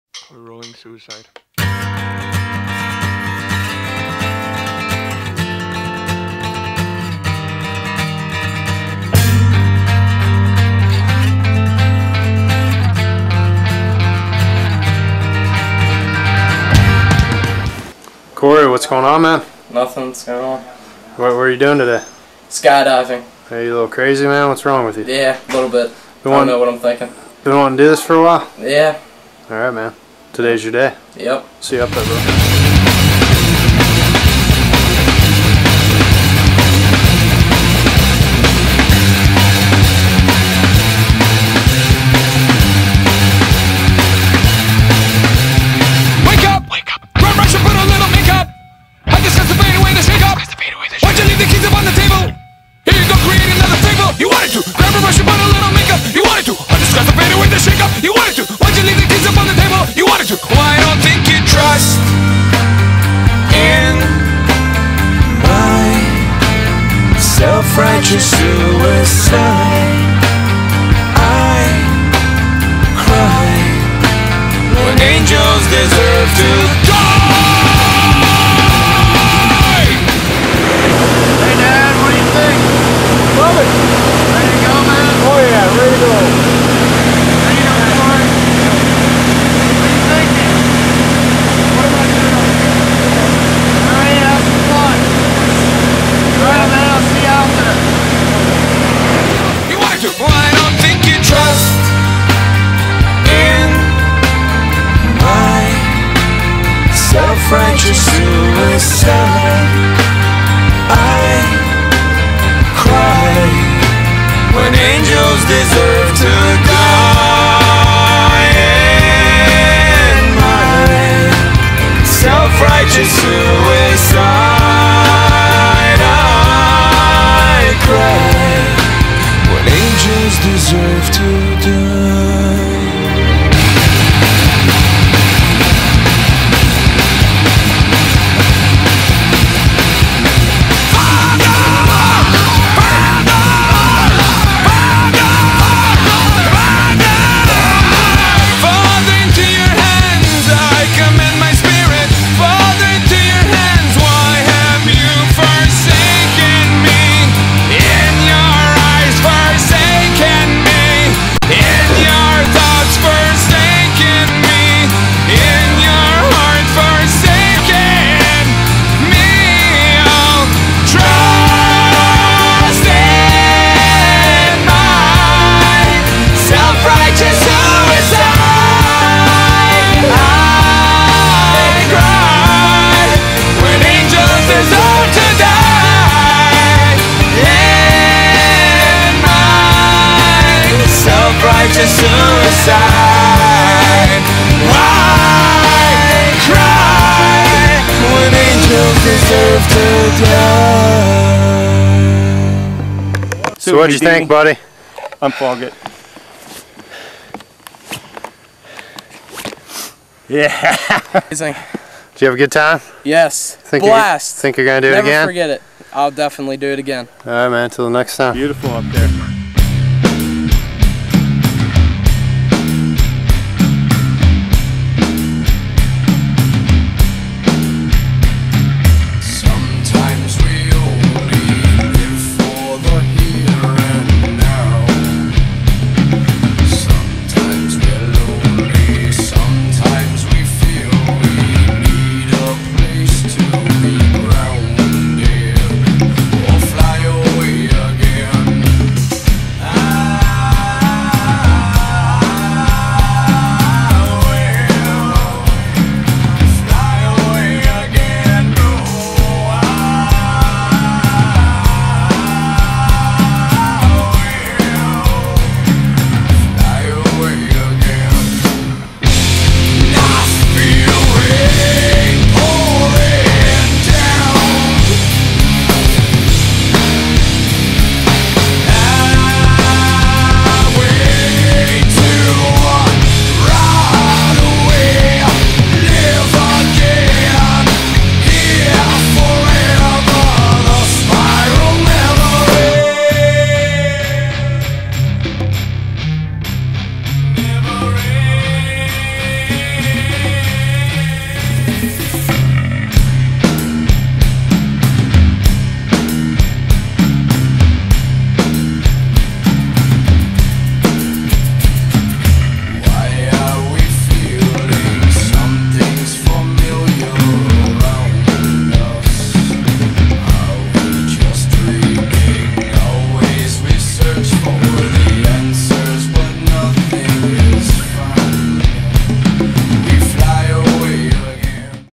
Cory Jr SKYDIVE
Cory Jrs first skydive mission!